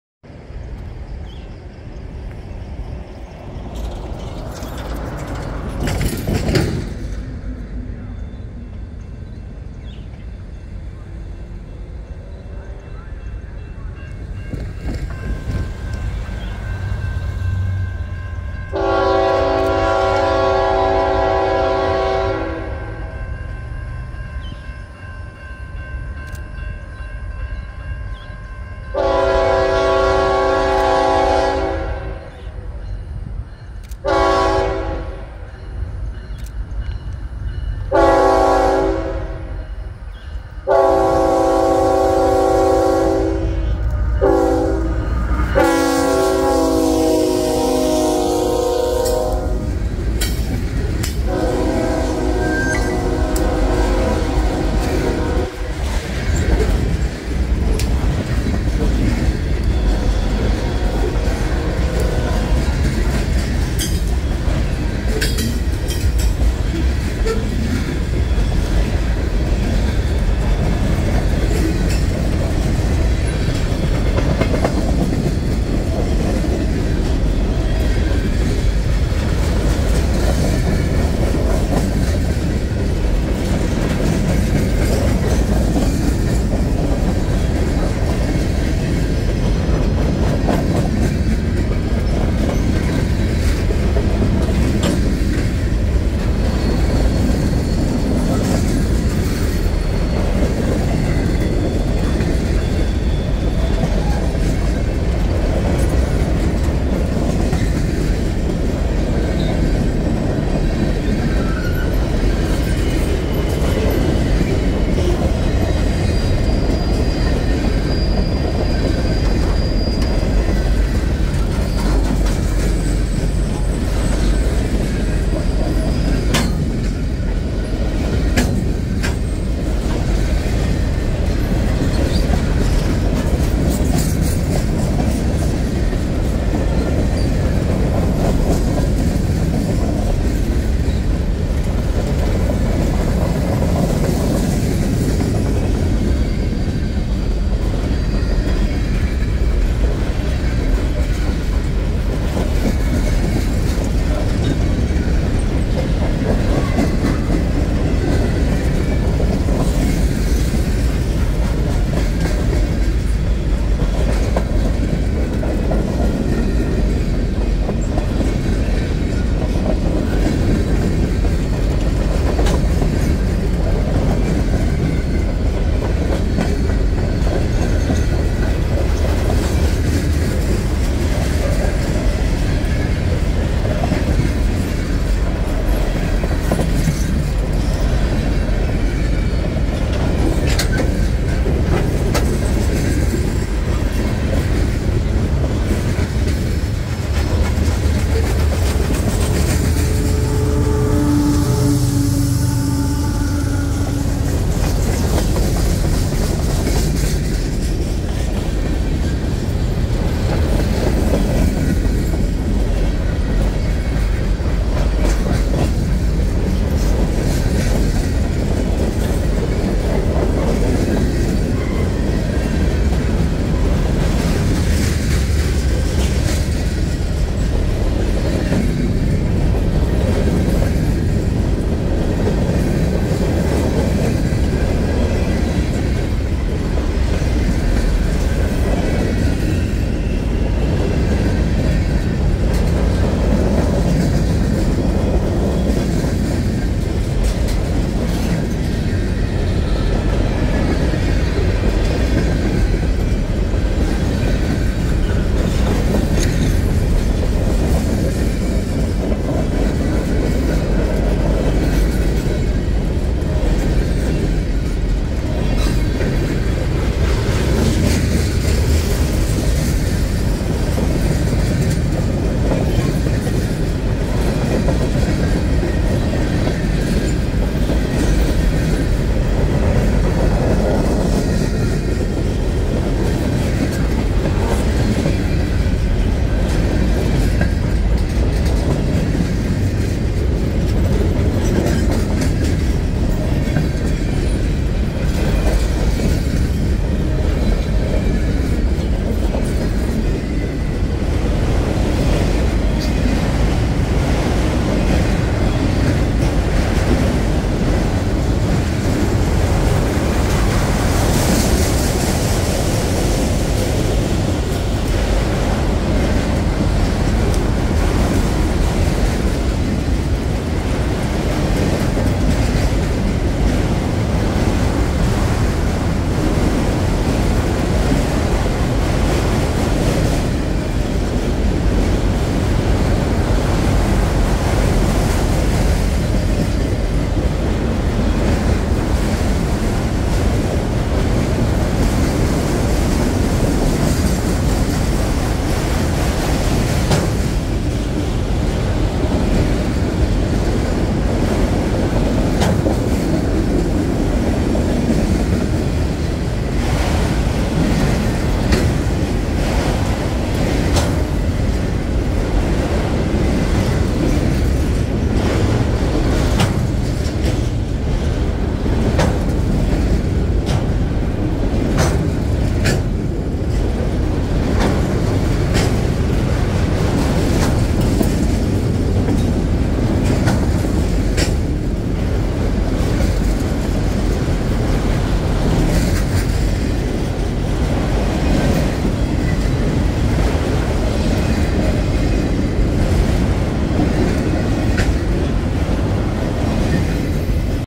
A Very Late M442-24 In Plant City 3/25/23
Filmed at Lake Wales Junction, the previous days' 442 was so late it ran the next morning.